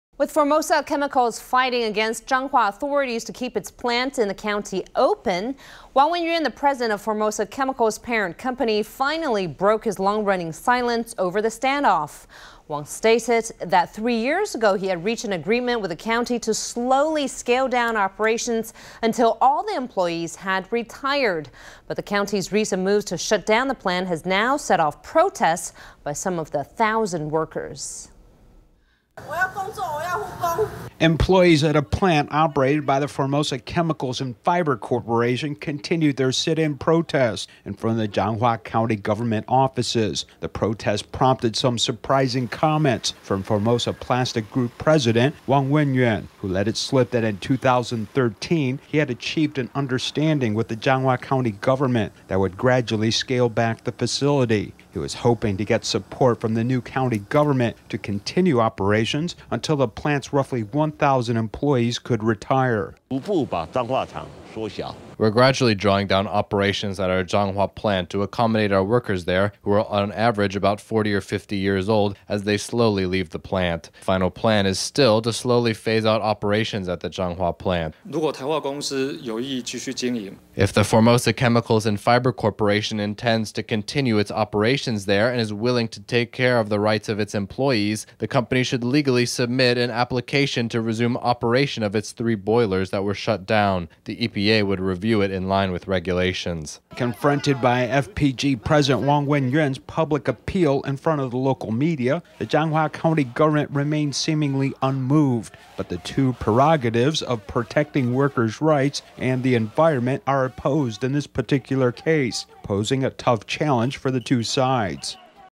Formosa Plastics pleads with Changhua County to keep chemical plant open
With Formosa Chemicals fighting against Changhua authorities to keep its plant in the county open, Wang Wen-yuan, the president of Formosa Chemicals'' parent company, finally broke his long-running silence over the standoff. Wang stated that three years ago he had reached an agreement with the county to slowly scale down operations until all the employees had retired. But the county''s recent moves to shut down the plant has now set off protests by some of the 1,000 workers. Employees at a plant operated by the Formosa Chemicals and Fibre Corporation continued their sit-in protest in front of Changhua County Government offices. The protest prompted some surprising comments from Formosa Plastics Group President Wang Wen-yuan, who let it slip that in 2013, he had achieved an understanding with the Changhua County Government that would gradually scale back the facility. He was hoping to get support from the new county government to continue operations until the plant’s roughly 1,000 employees could retire.Wang Wen-yuanFPG PresidentWe’re gradually drawing down operations at our Changhua plant to accommodate our workers there, who are on average about forty or fifty years old, as they slowly leave the plant. The final plan is still to slowly phase out operations at the Changhua plant. Our existing plants producing cosmetics and lithium batteries are also moving in that direction. Lin De-yuChanghua County Dept. of InformationIf the Formosa Chemicals and Fibre Corporation intends to continue its operations there and is willing to care of the rights of its employees, the company should legally resubmit an application to resume operation of its three boilers that were shut down. The EPA would review it in line with regulations.Confronted by FPG President Wang Wen-yuan’s public appeal in front of local media, the Changhua County government remained seemingly unmoved. But the two prerogatives of protecting workers’ rights and the   environment are opposed in this particular case, posing a tough challenge for the two sides.